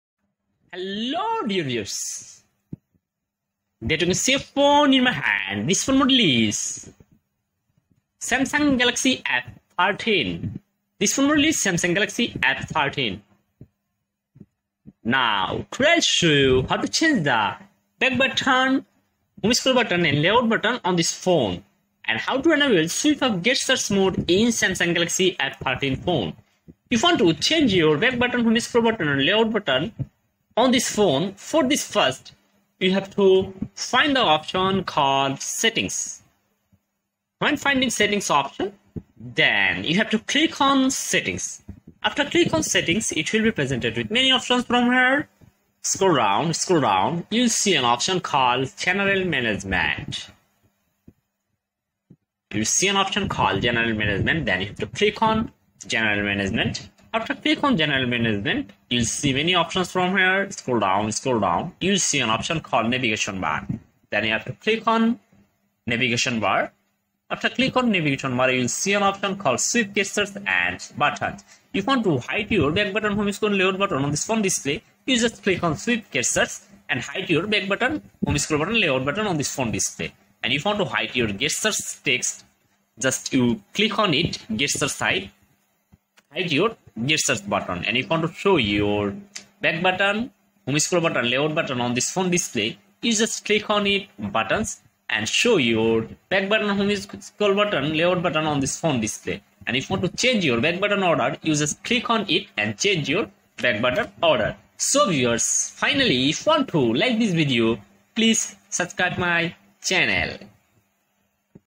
Samsung galaxy F13 back button not show // Back button change setting
This phone model is samsung galaxy f13. now today i will show you how to change navigation bar in Samsung galaxy f13 phone. and i will also show you today how enable swipe gesture in Samsung galaxy F13 phone.